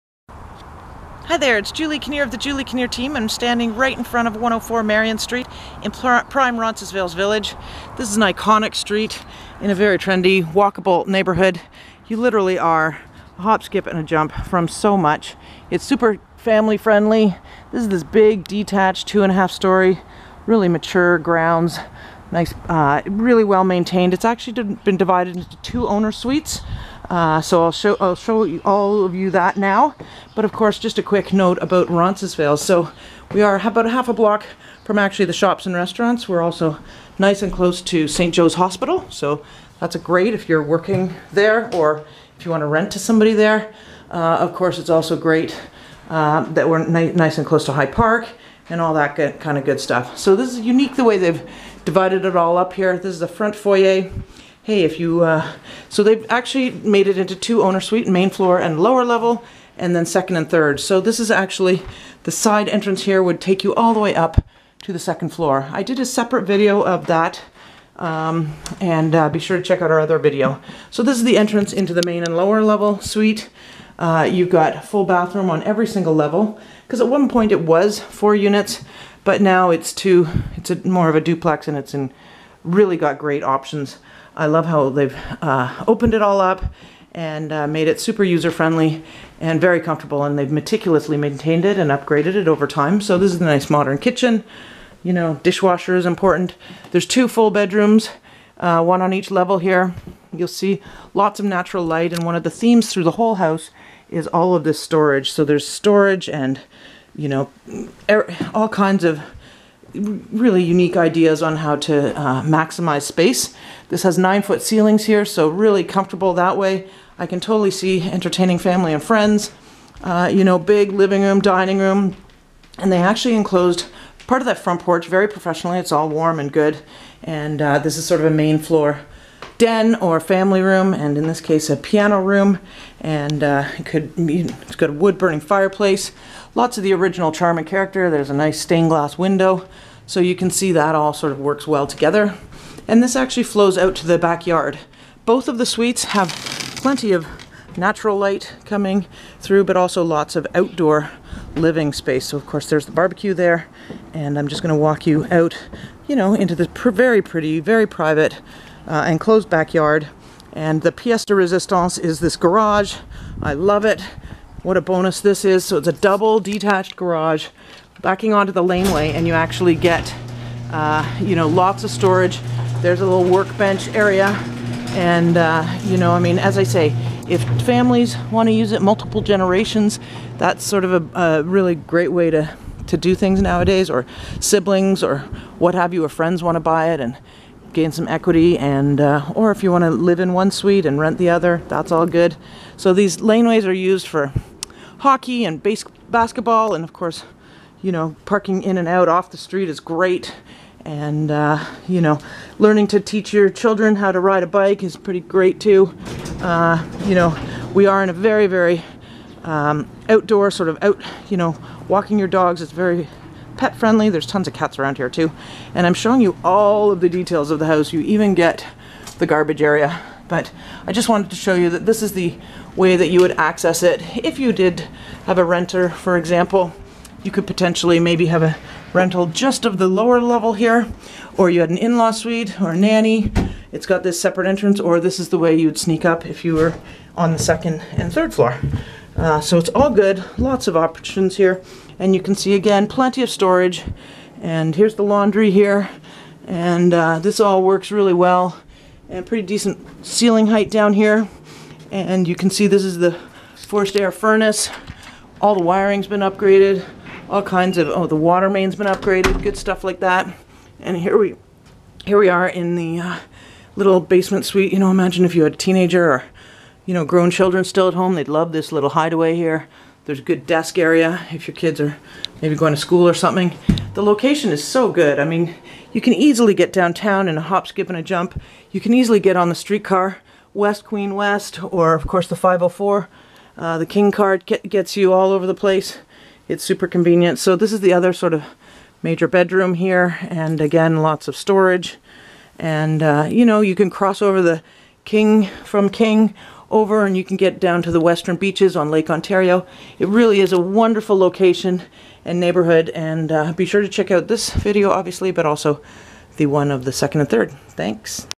104 Marion Street - Main & Lower Level | West Toronto - Roncesvalles
104 Marion Street | West Toronto - Roncesvalles, Toronto Real Estate For Sale

Opportunity Knocks On The Door of This Duplex in Roncy!

Incredible investment set on one of the iconic streets in Roncesvalles! Space galore throughout this well upgraded and meticulously maintained detached 2.5 storey home with oversized double car garage off lane - currently set up with 2 2-level owner suites!

25 x 120 foot lot, 3030 sf including lower level, 4 + 1 bedrooms, 2 renovated kitchens, 4 full baths (one on every level) and solid mechanicals.

Summer time is almost here and there is plenty of outdoor space for each suite to enjoy! Large covered front porch, 2 separate balconies on the second floor and a private balcony on the third! Fully fenced garden and stone patio for entertaining family/friends and kids/pets play!

Potential options for a multitude of buyers as home was at one point 4 units:
- Live in one unit while gaining income from the other
- Convert to a single family dwelling with a separate basement suite for income or nanny.
- Separate side entrance to lower level for a 3rd separate income suite
- Siblings/friends/multi-generations can pool their money to be in the market while having their own space and gaining equity - the units can be either connected or separated with an easy adjustment.
- Investors looking for a prime piece of real estate in a hot location

Inclusive family-friendly neighbourhood with lots of kids on the street and good schools (plenty in French Immersion), makes this a delightful home and 'hood' to live and relish all the area amenities.

Priceless location (with 2 car parking!) on a pretty street in Roncesvalles Village - walk to all the action on Roncy and Queen St. West shops, restaurants, streetcar, Lake Ontario western beaches, St Joe’s Hospital and High Park.

Julie Kinnear, Jennifer Palacios, Holly Chandler, Tyler Delaney, Lisa Taliano, Kathy Marinkovic, Lisa Lupynec - Sales Representatives